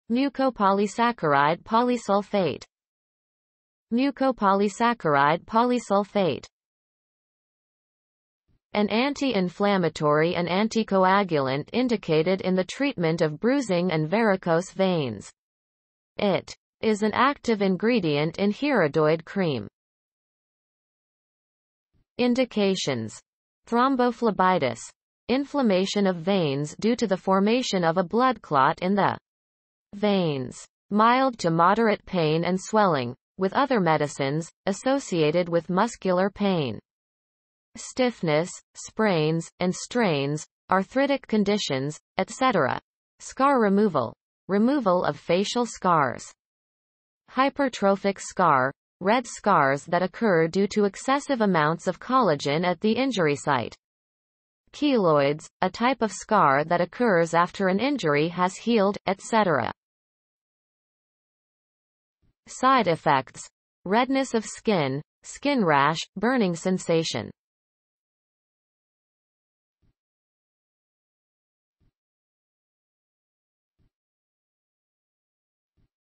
mucopolysaccharide polysulfate
An anti-inflammatory and anticoagulant indicated in the treatment of bruising and varicose veins.
It is an active ingredient in Hirudoid cream.

(Indications)
• Thrombophlebitis: Inflammation of veins due to the formation of a blood clot in the veins.
• Mild to moderate pain and swelling (with other medicines): Associated with muscular pain, stiffness, sprains, and strains, arthritic conditions, etc.
• Scar removal: Removal of facial scars, hypertrophic scar (red scars that occur due to excessive amounts of collagen at the injury site), keloids (a type of scar that occurs after an injury has healed), etc.

(Side effects)
• Redness of skin, skin rash
• Burning sensation